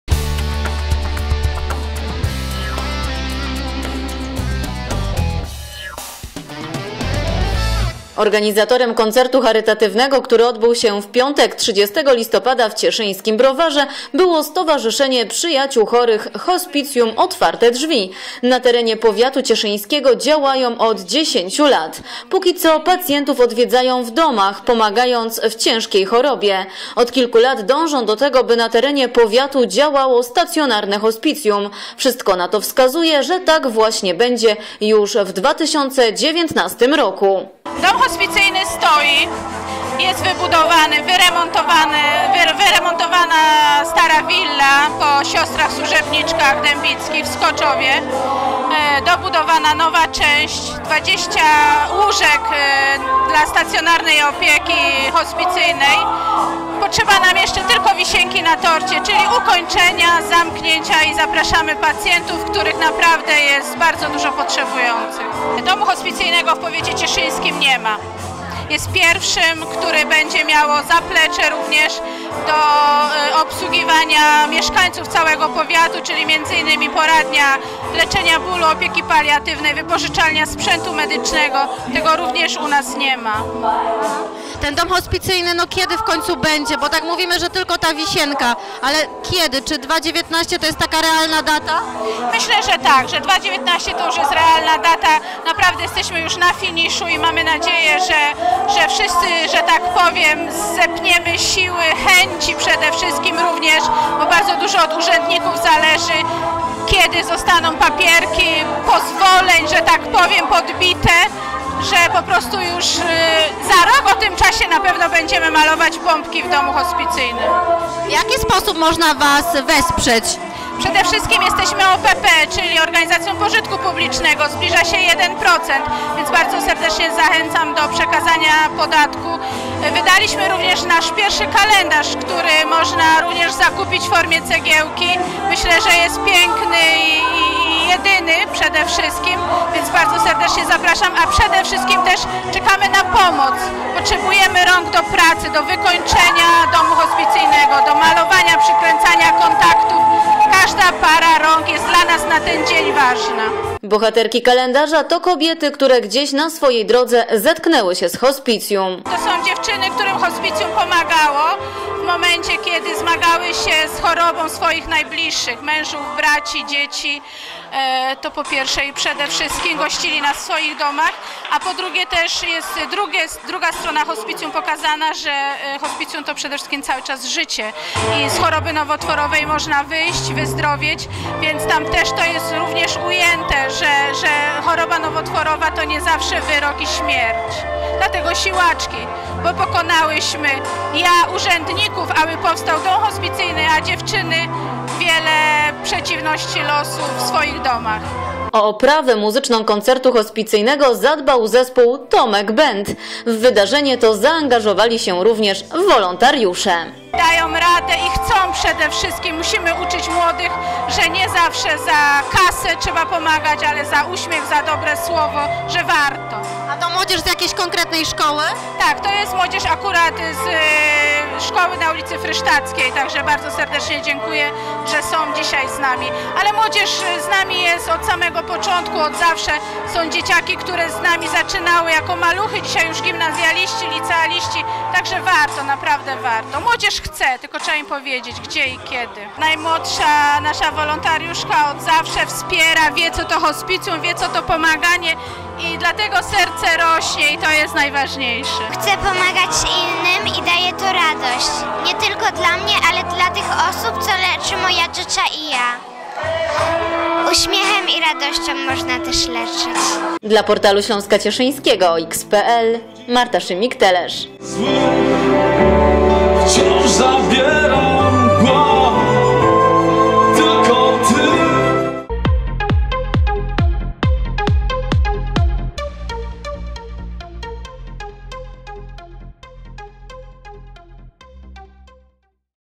Koncert Hospicyjny w cieszyńskim Browarze
W piątek (30.11) w cieszyńskim Browarze Zamkowym odbył się Koncert Hospicyjny.
┌ Rozwiń Opis

 Wydarzenie to połączono z premierą pierwszego kalendarza hospicyjnego na 2019 rok zatytułowanego "Siłaczki".